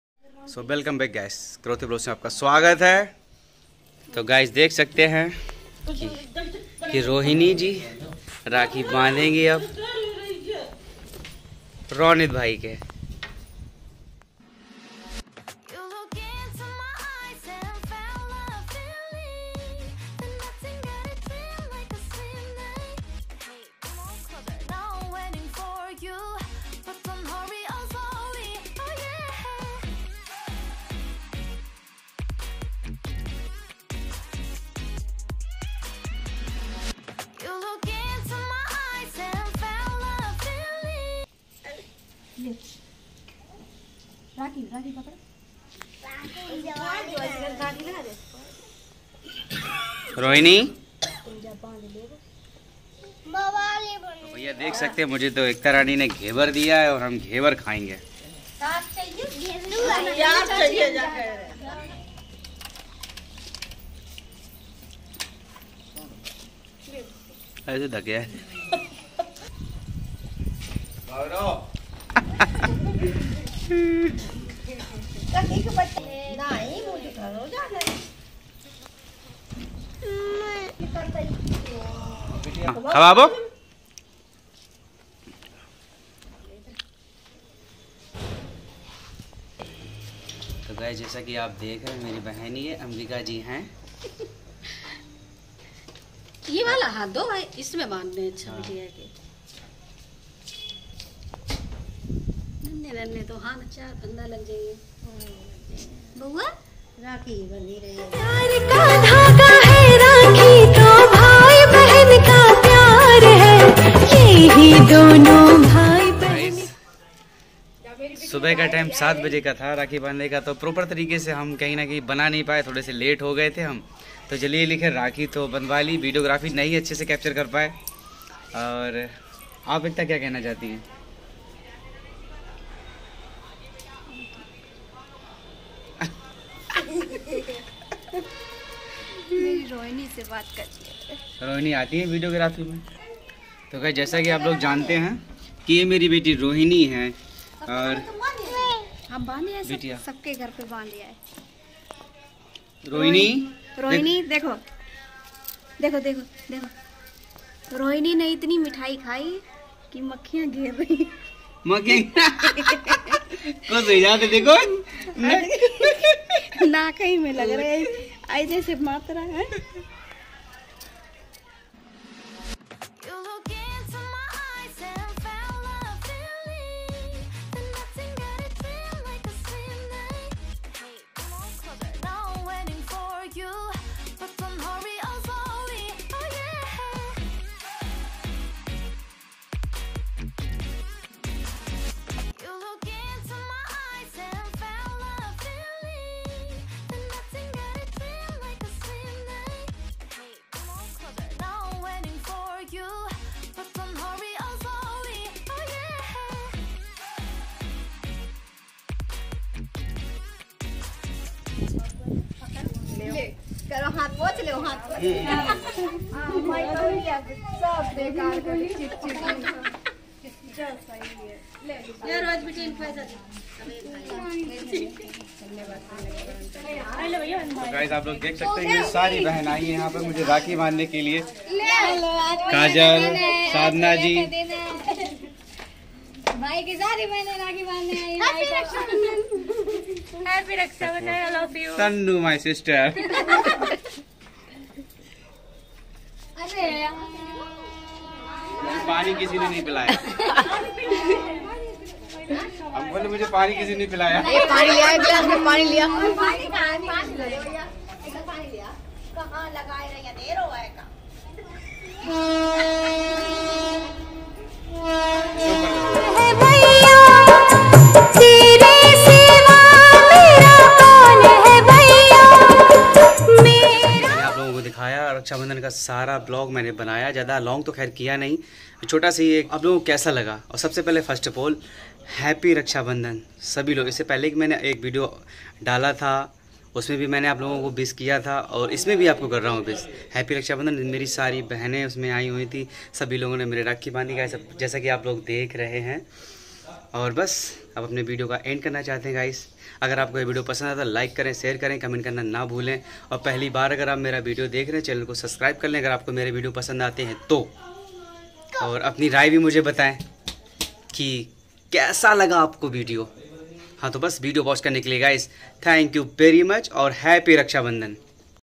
मेरी प्यारी बहने || मुझे राखी बांधने के लिए आए || रक्षा बंधन का Latest Video
video highlights
video public

ekta Karotiya
Sadhana
tannu
Jyoti
Ambika kanpur
Shivani
chottu

Rohit Karotiya Vlogs channel
Kannauj Uttar Pradesh and